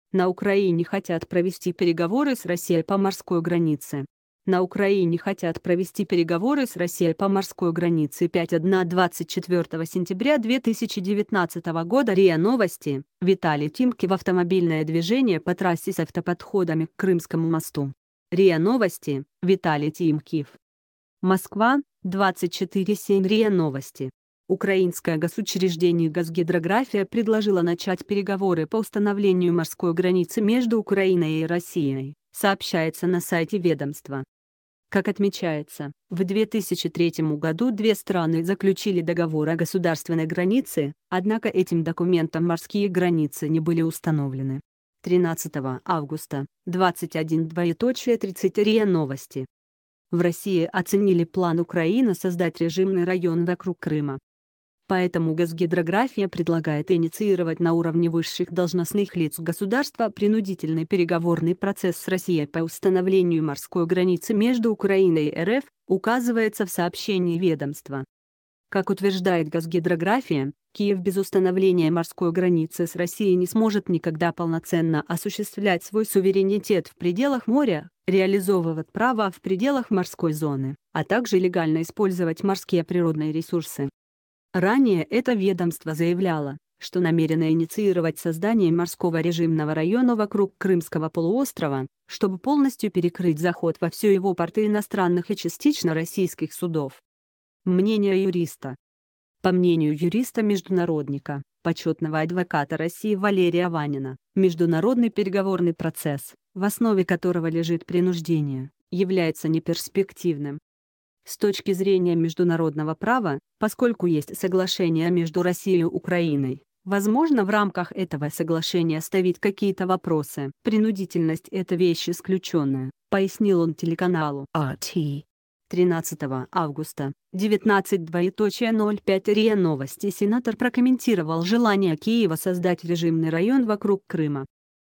На Украине хотят провести переговоры с Россией по морской границе
На Украине хотят провести переговоры с Россией по морской границе На Украине хотят провести переговоры с Россией по морской границе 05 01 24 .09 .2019 РИА Новости Виталий ТимкивАвтомобильное движение по трассе с автоподходами к Крымскому мосту РИА Новости Виталий Тимкив МОСКВА, 24 сен РИА Новости . Украинское госучреждение Госгидрография предложило начать переговоры по установлению морской границы между Украиной и Россией, сообщается на сайте ведомства . Как отмечается, в 2003 году две с